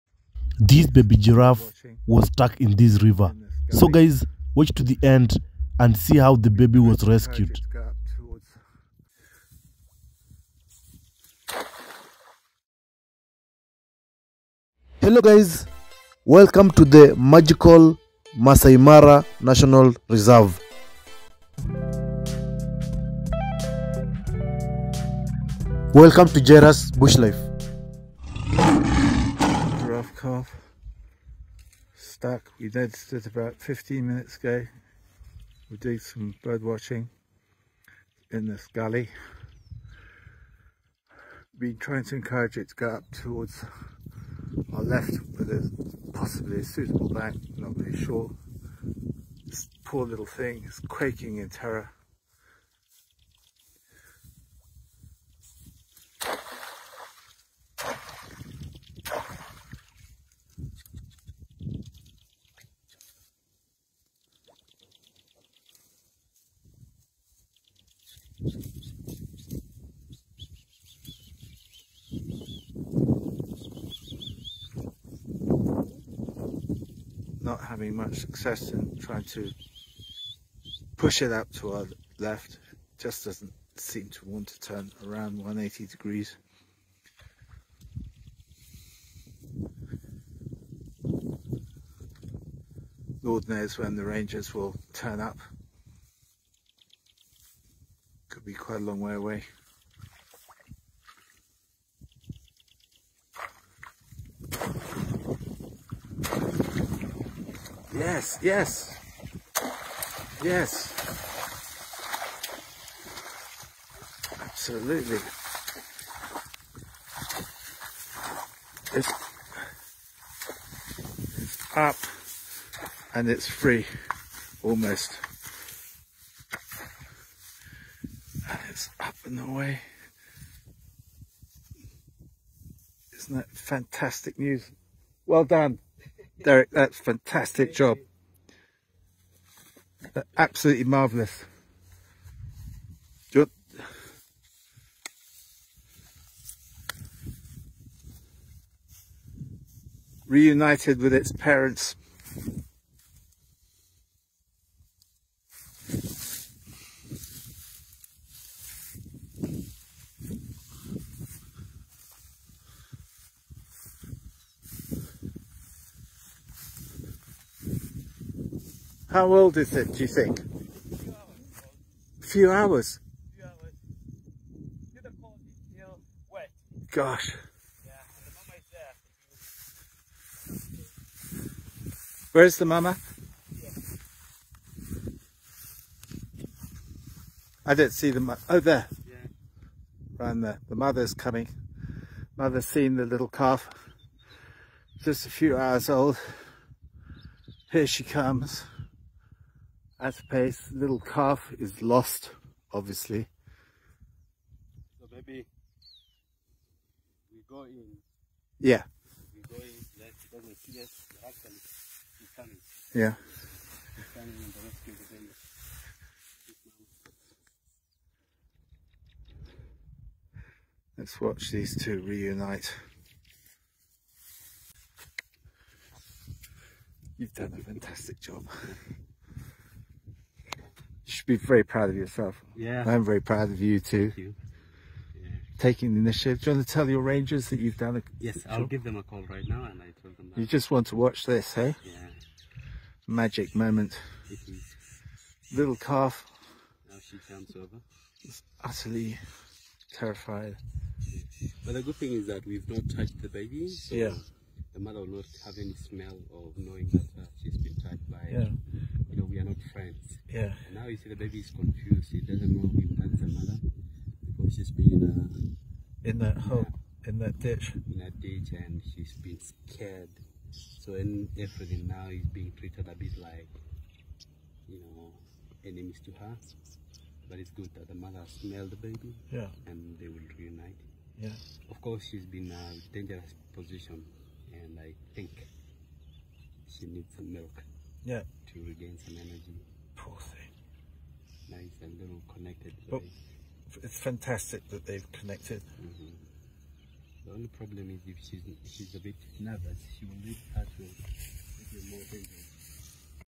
Giraffe newborn baby Rescued from Crocodile invaded River _ Giraffe vs Man in Africa
Newborn baby Giraffe Rescued from a Crocodile invaded River in Masai Mara National Reserve giraffe Giraffe rescue Rescued